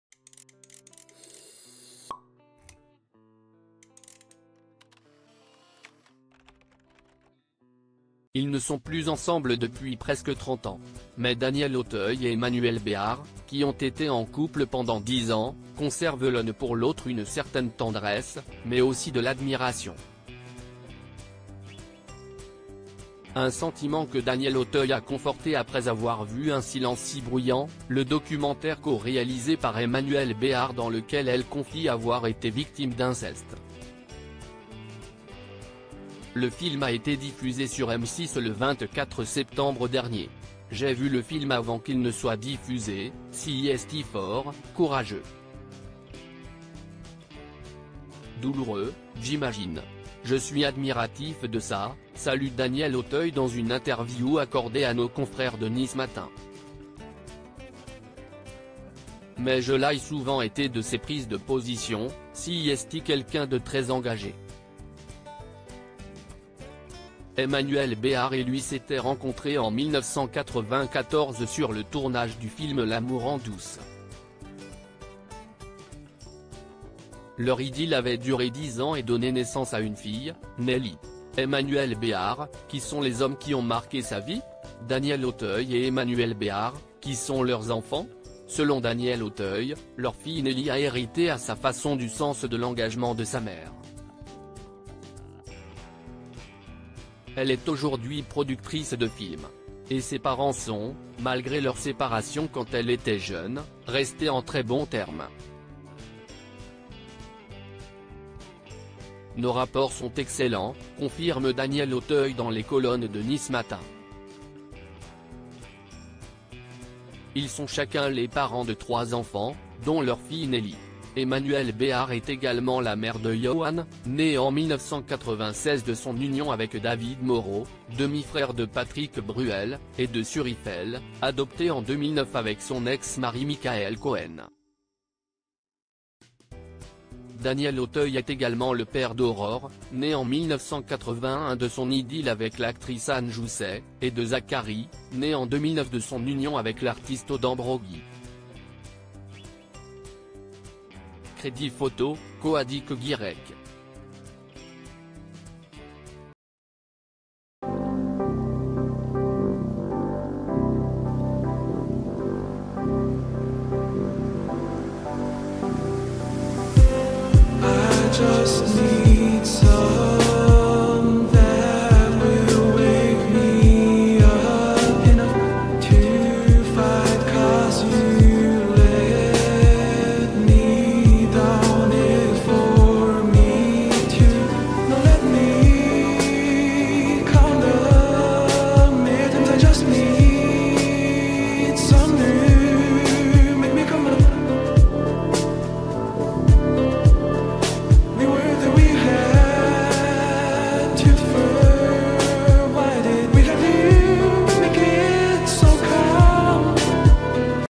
Emmanuelle Béart victime d’inceste, son ex Daniel Auteuil réagit à son documentaire “C’est fort, c
Emmanuelle Béart victime d’inceste, son ex Daniel Auteuil réagit à son documentaire : “C’est fort, courageux”